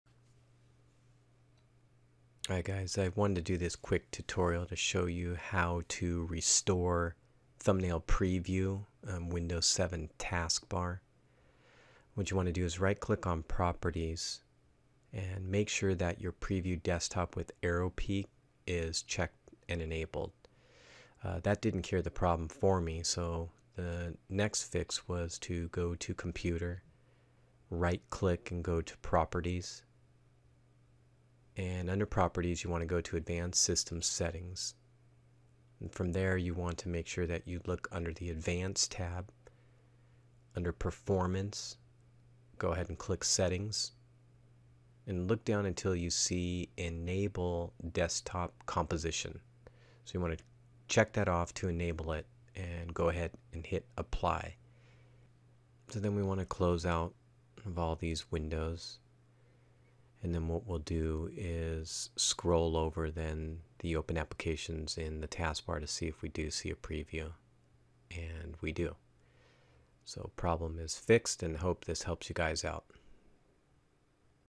Restore Thumbnail Preview In Windows 7 Task Bar
Step by step video instructions on how to restore thumbnail previews in windows 7 task bar